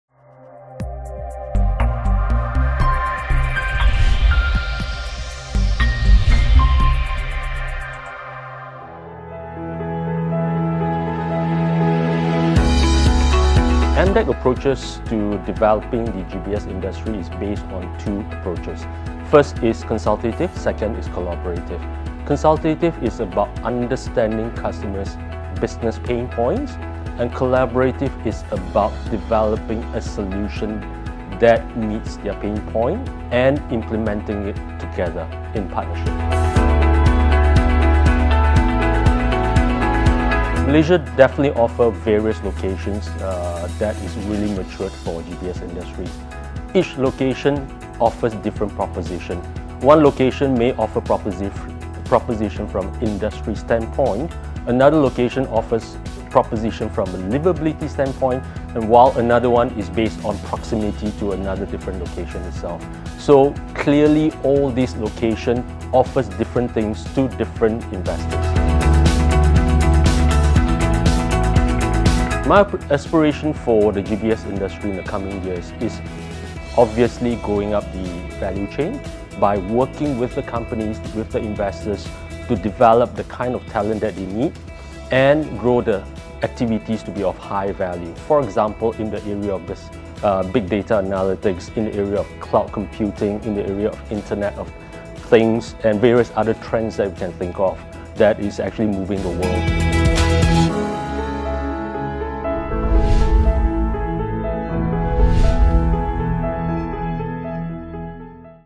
GBS: Malaysian Locations Have Clear and Distinct Value Propositions
Hew Wee Choong – VP, Global Business Services (GBS) of Malaysia Digital Economy Corporation (MDEC) – speaks to the GBS TV about Malaysian Locations and his agency’s role in nurturing and growing the sourcing ecosystem.